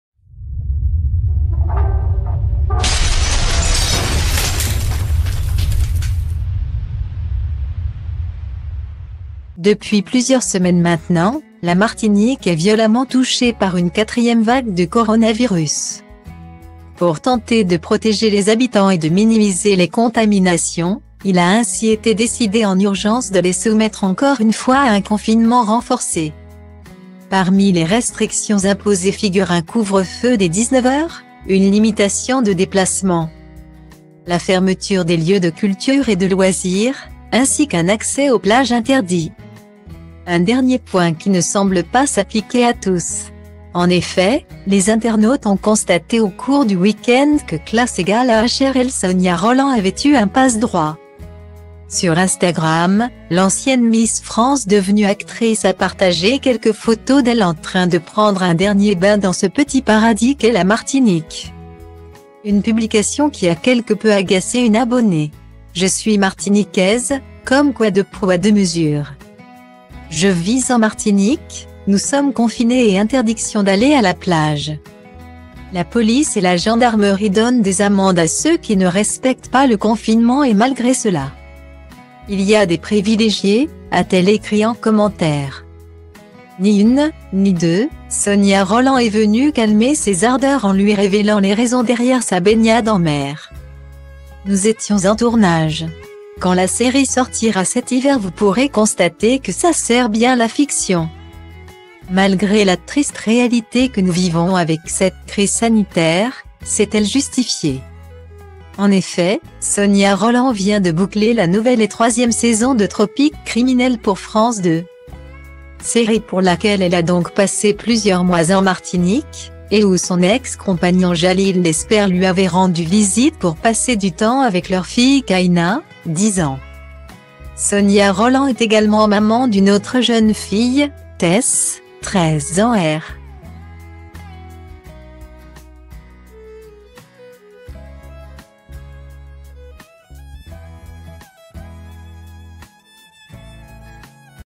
Sonia Rolland en Martinique et "privilégiée" ? mise au point après une accusation
Depuis plusieurs semaines maintenant, la Martinique est violemment touchée par une quatrième vague de coronavirus.Pour tenter de protéger les habitants et de minimiser les contaminations, il a ainsi été décidé en urgence de les soumettre encore une fois à un confinement renforcé.Parmi les restrictions imposées figurent un couvre feu dès 19h, une limitation de déplacements,